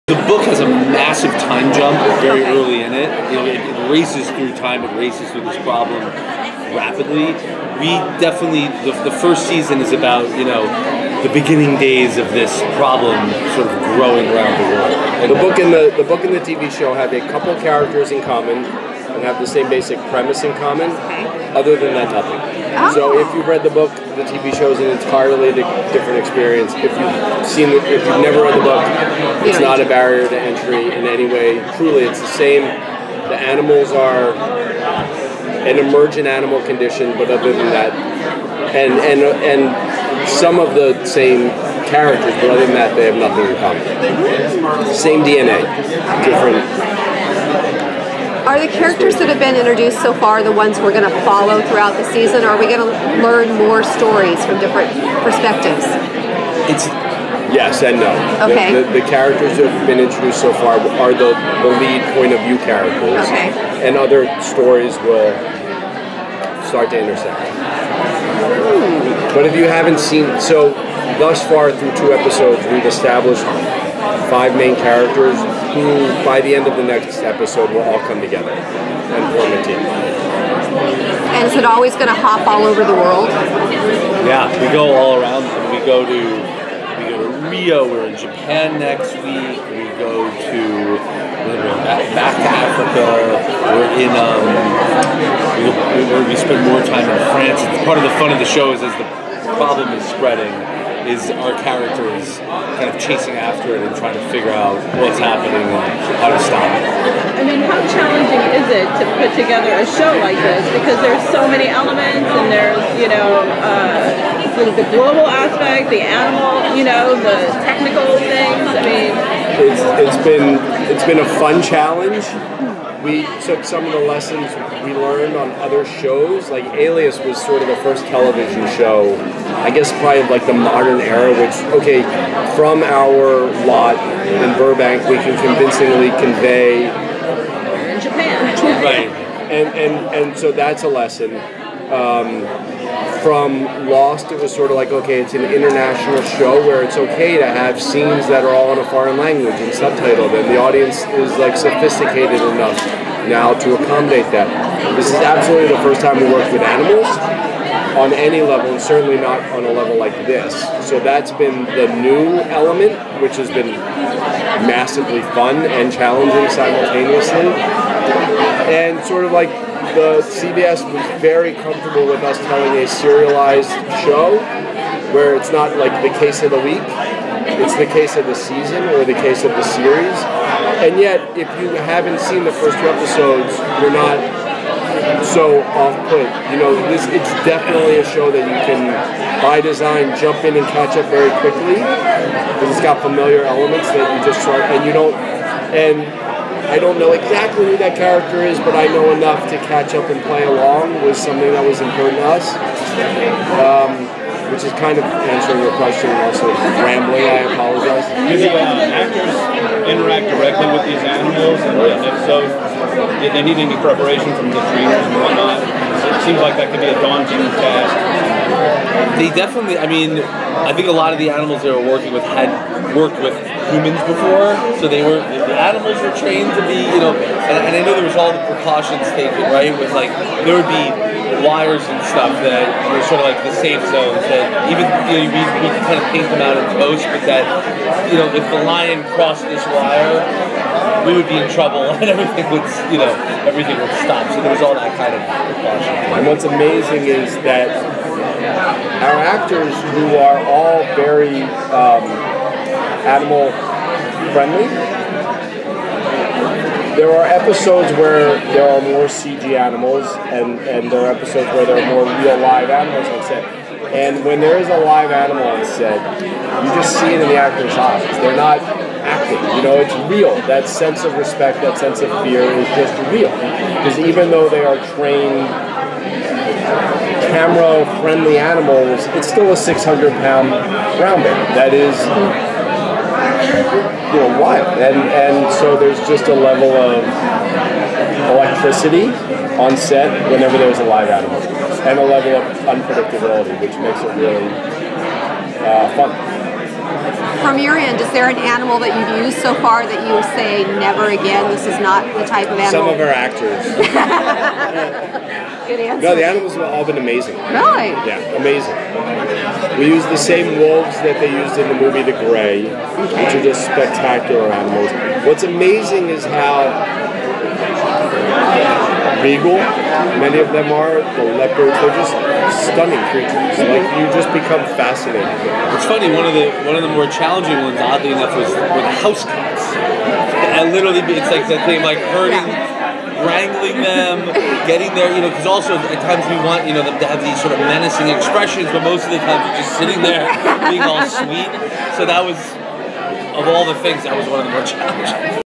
Zoo - Josh Appelbaum and Jeff Pinkner Interview, Season 1
Zoo executive producers Josh Appelbaum and Jeff Pinkner discuss the source material, the animals, and what viewers can expect from season one.   The interview took place at the 2015 San Diego Comic Con.